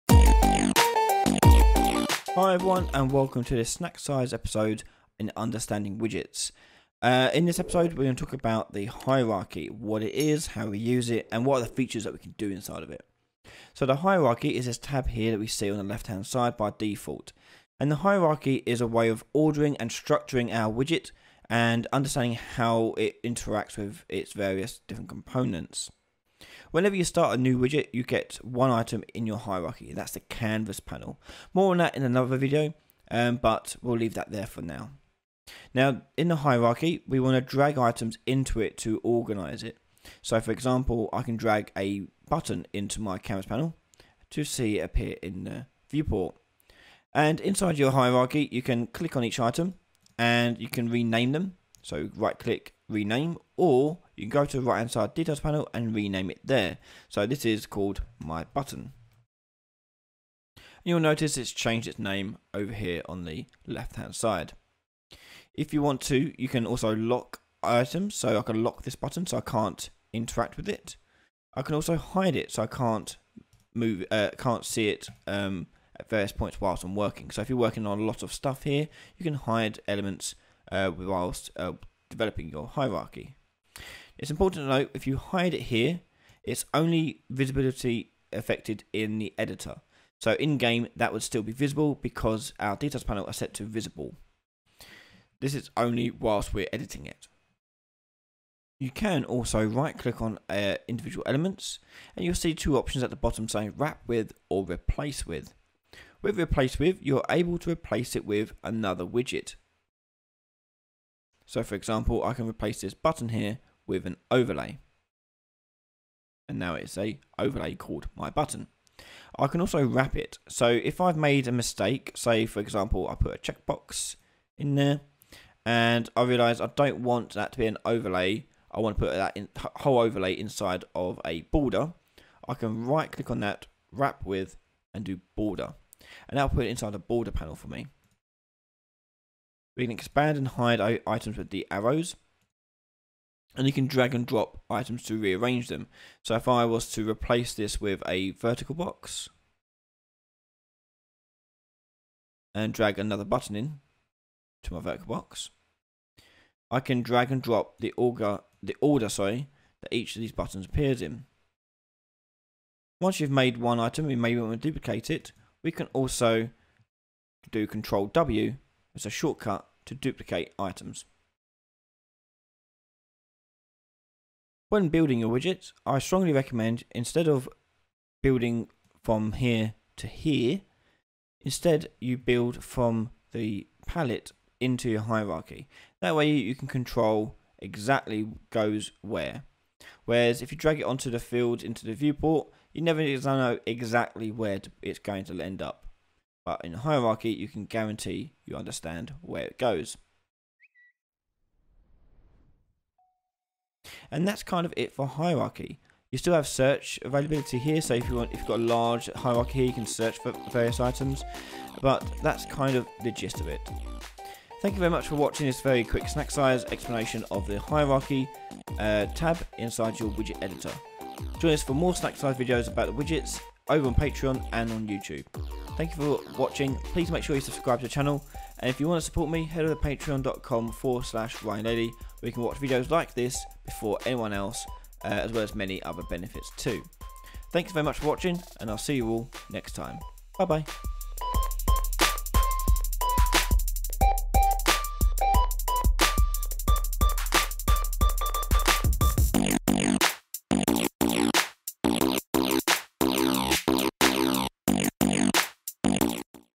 Unreal Engine 4 SnackSize - Hierarchy (Widgets)
For those completely new to Unreal, follow these short explanation videos to get a better grasp of blueprints and making games. This episode we take a look at the hierarchy panel in the widget editor.